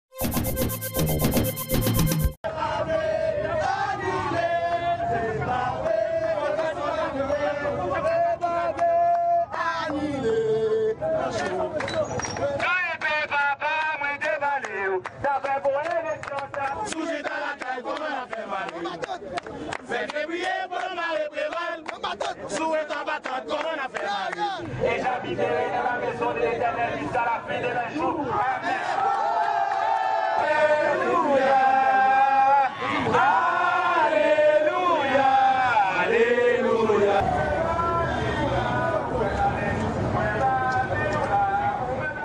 Haïti: manifestation contre les fraudes à la présidentielle
Une manifestation à l'appel d'une douzaine de candidats malheureux à la présidentielle demandait dimanche 23 janvier l'annulation pure et simple du premier tour. Durée: 00:42